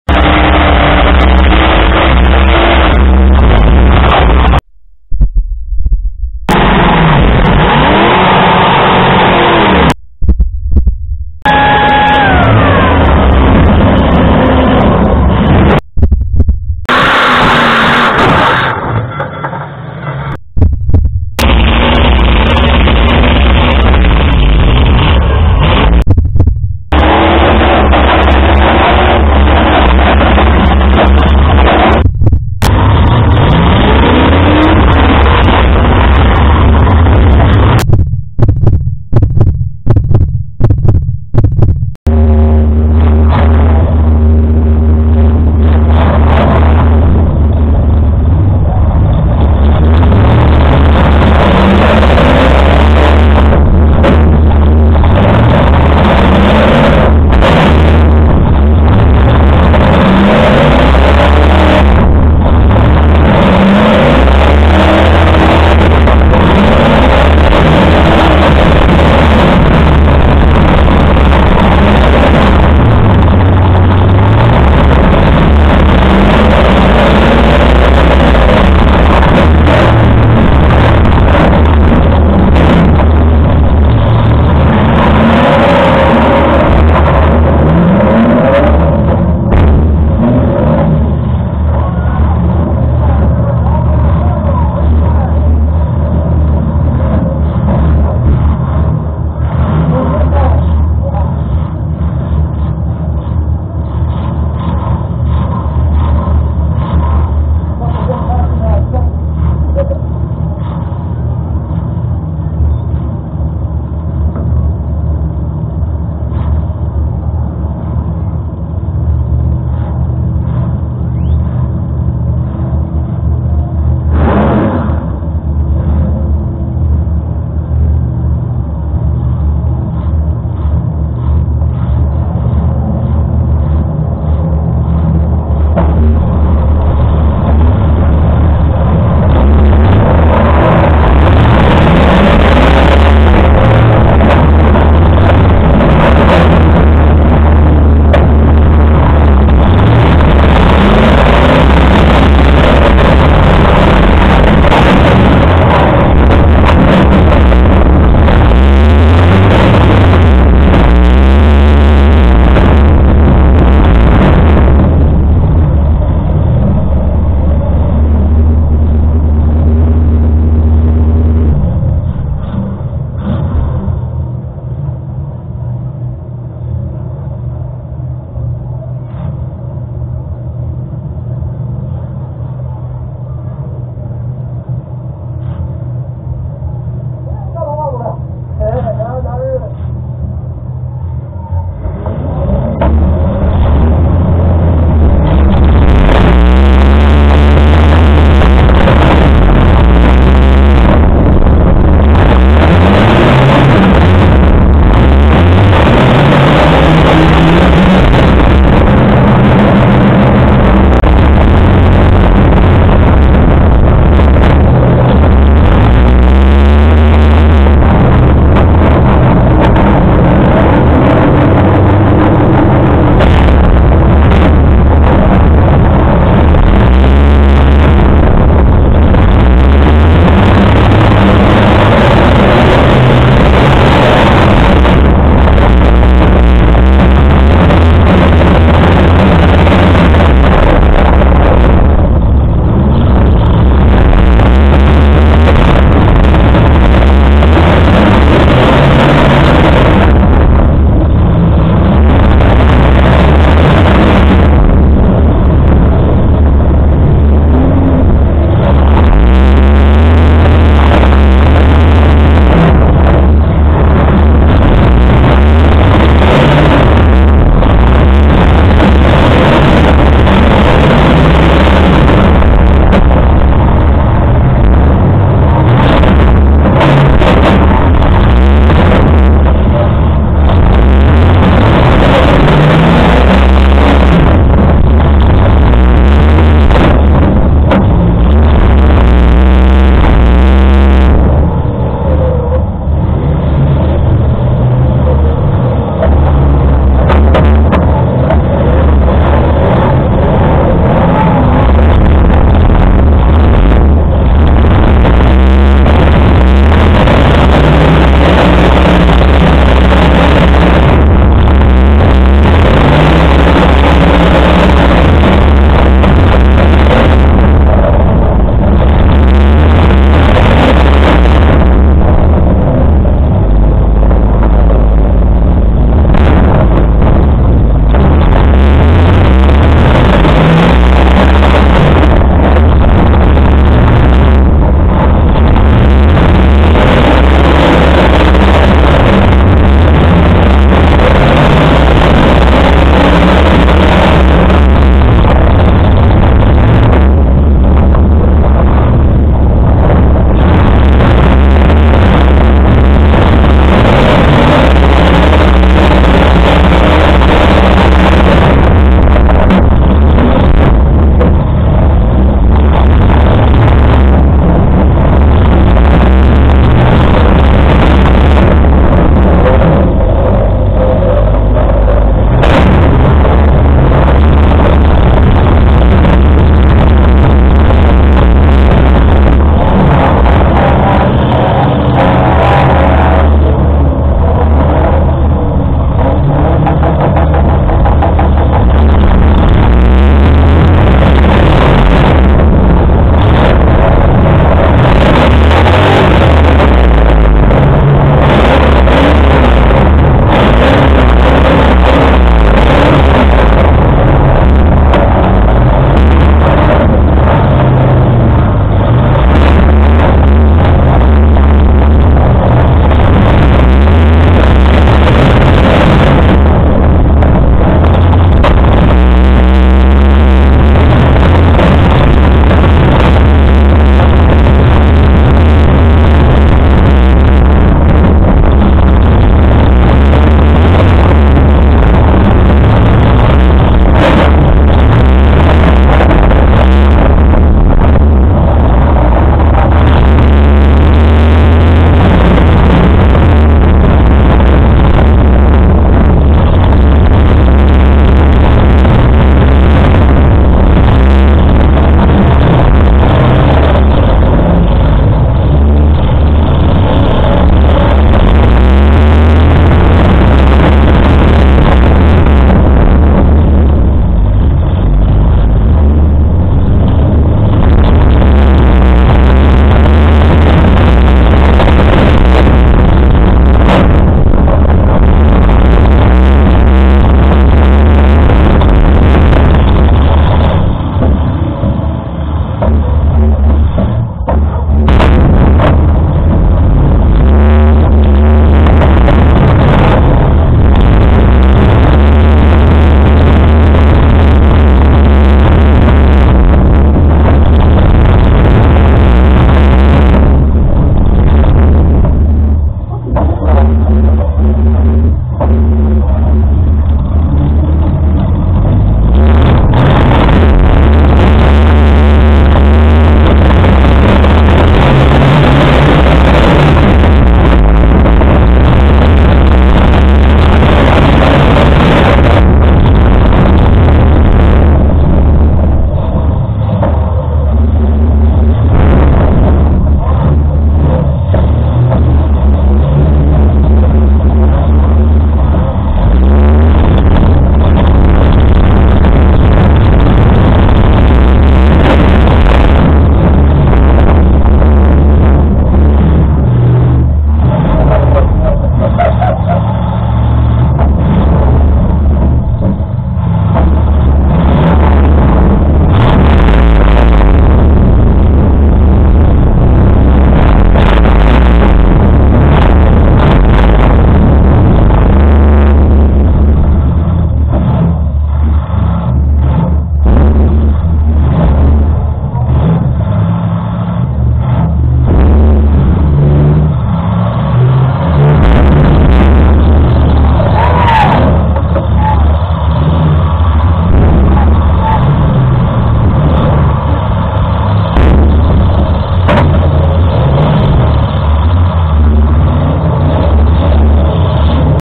Crazy Taxi 116 Chapaign, IL 2023
Anthony Ashby in the 116 Crazy Taxi in the 80s wire class at the Champaign, IL county fair 7/22/23